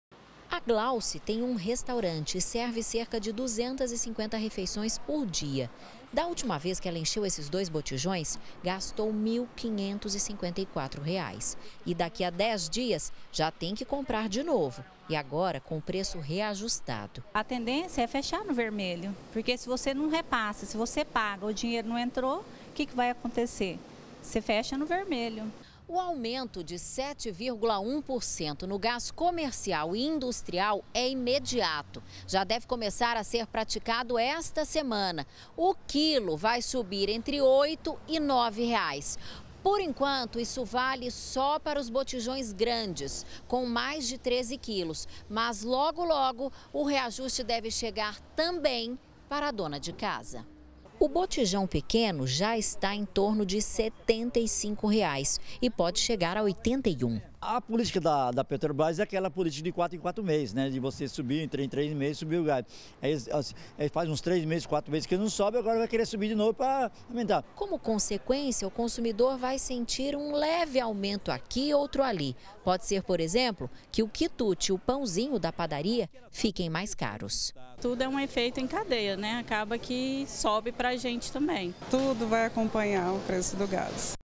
GR - Gás: Preço sobe mais uma vez - 08-05-2018
A Petrobras reajustou o preço do gás industrial. E por tabela, a gente pode esperar um aumento em cascata. Muitos comerciantes devem repassar o custo pros consumidores. E tem mais. O próximo reajuste deve ser do botijão de cozinha. O preço em Goiânia deve passar de oitenta reais.